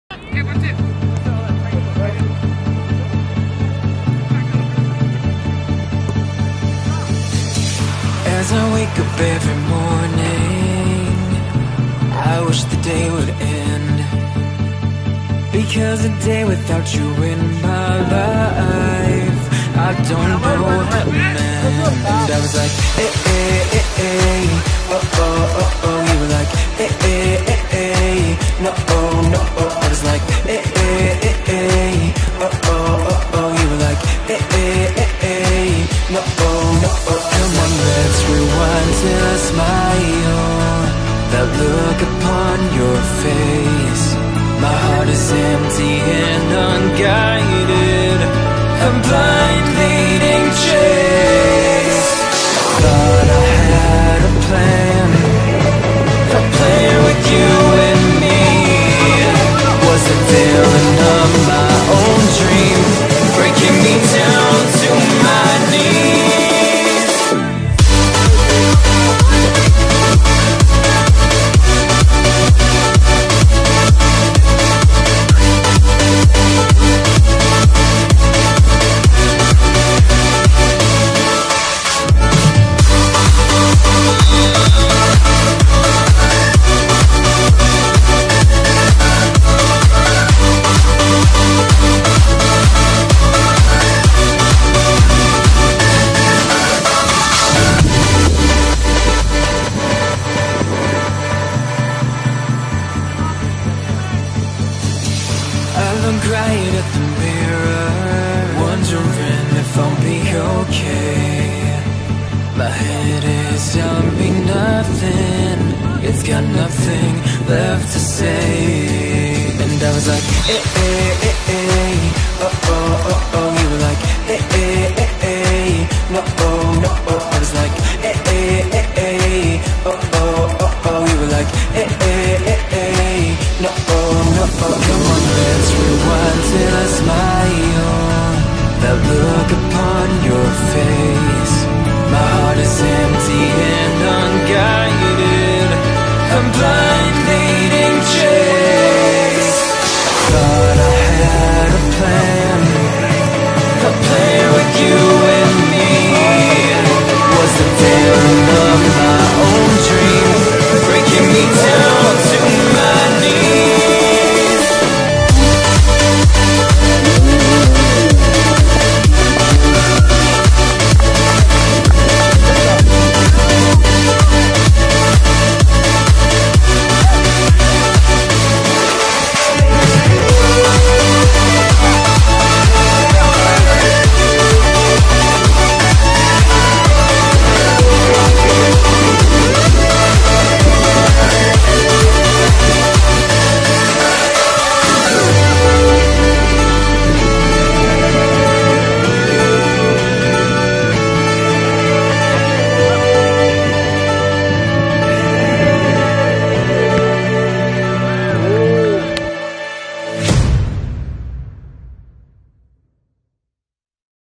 GAME HIGHLIGHTS - WE WON 11-2 | FDB Soccer
The opposition wasn't the best, but these games are helping me get fit again and recover from my groin injury. Need to take it slow to make sure my fitness comes back the proper way. I cannot push it super hard yet because I do not want to pull my groin again.

Also, no matter which level it is, playing the sport you love is always such a joy.